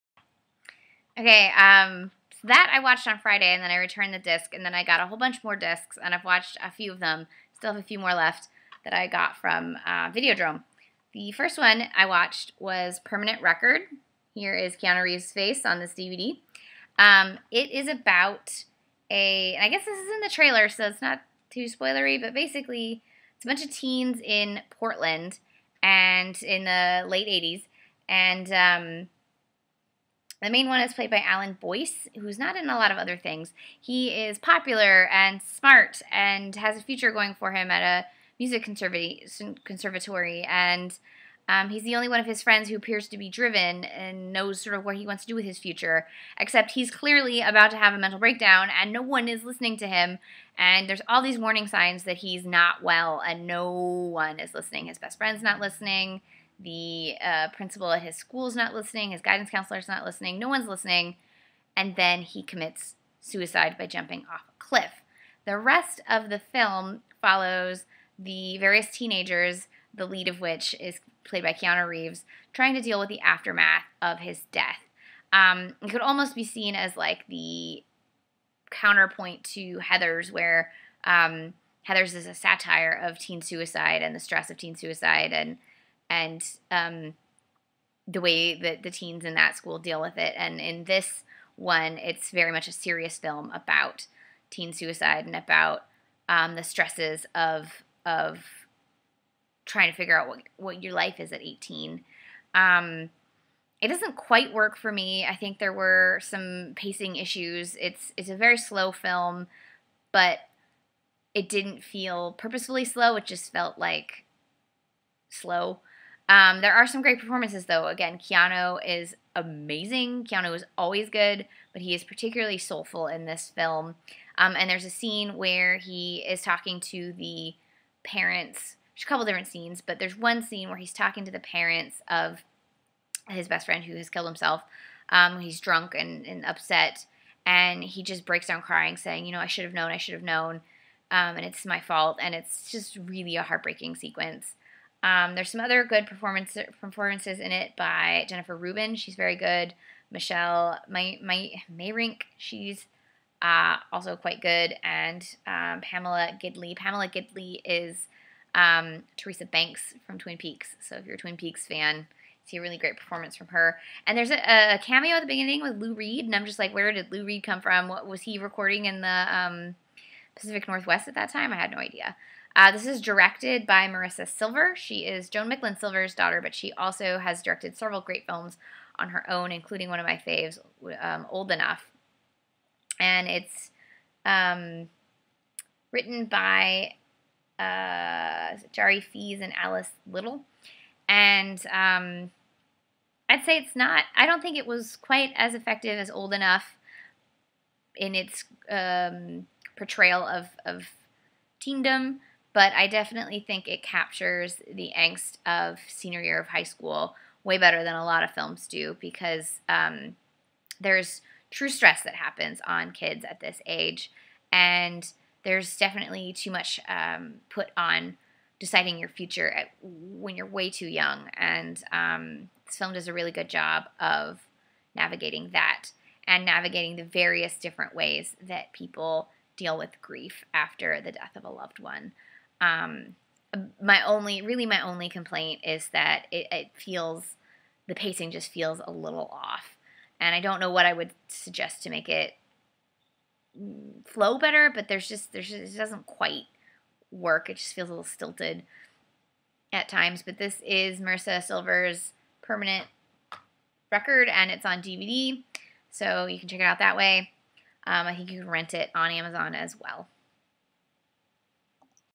oldfilmsflicker on Marisa Silver's "Permanent Record"
A video review of Marisa Silver's "Permanent Record" starring Alan Boyce, Jennifer Rubin, Michelle Meyrink, Pamela Gidley, and Keanu Reeves